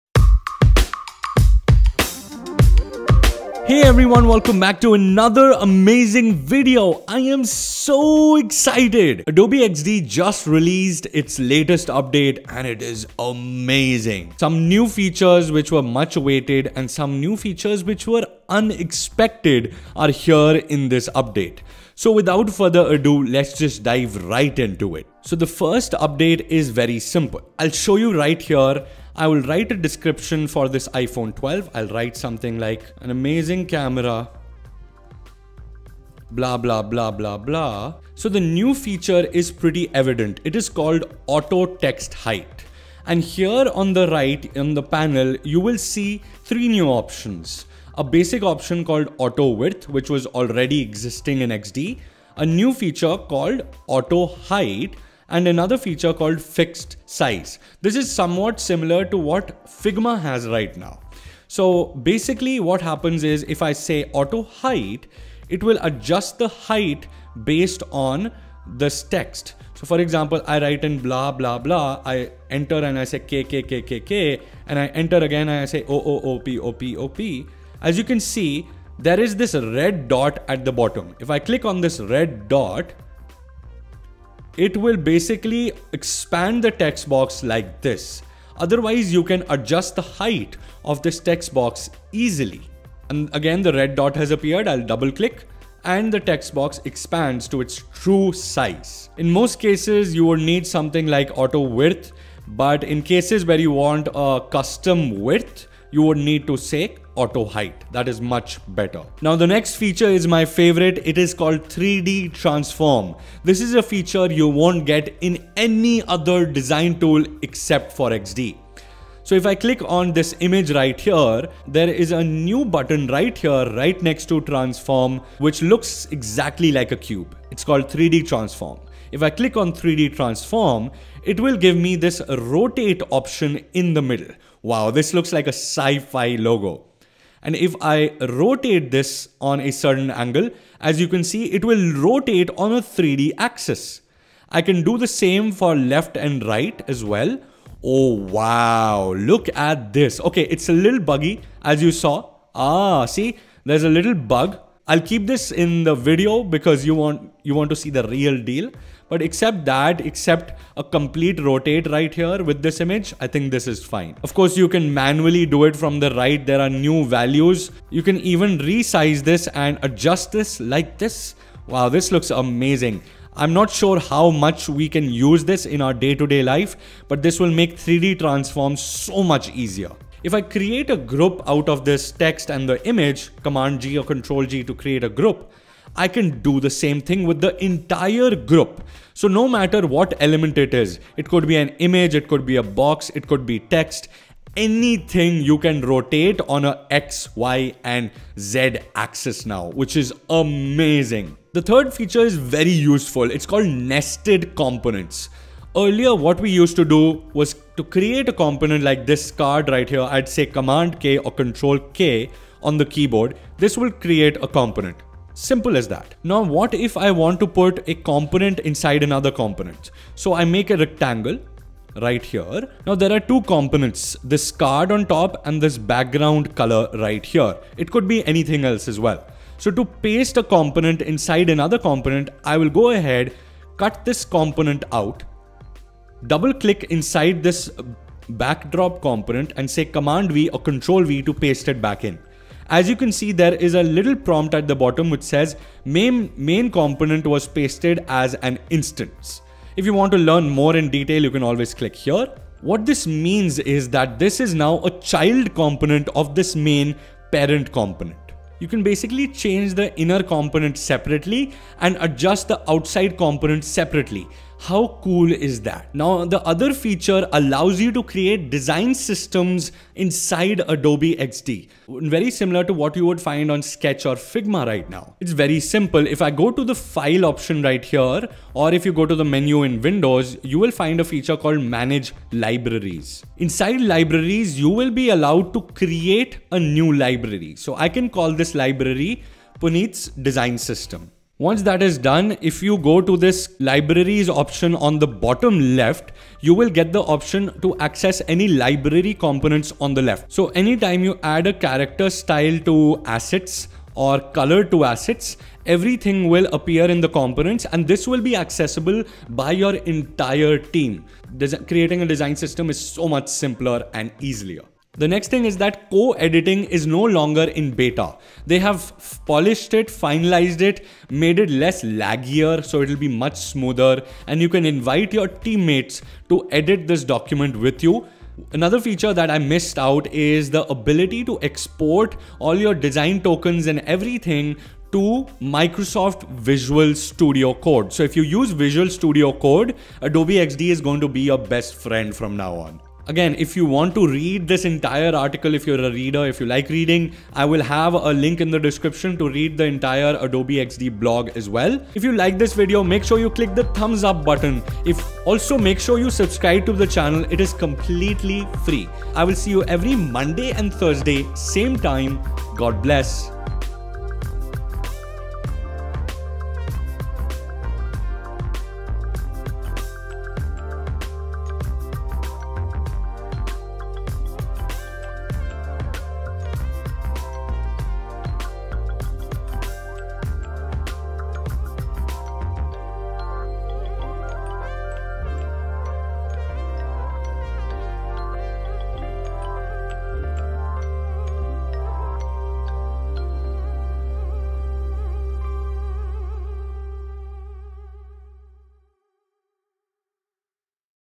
Adobe Xd October Update is Mind Blowing 🤯 | Adobe Max | Design Essentials
Today we're checking out an Amazing New Adobe Xd update! The October release of Xd has some amazing new features including nested components, auto text height and 3D transforms among many other new Adobe Xd features.

2. Blush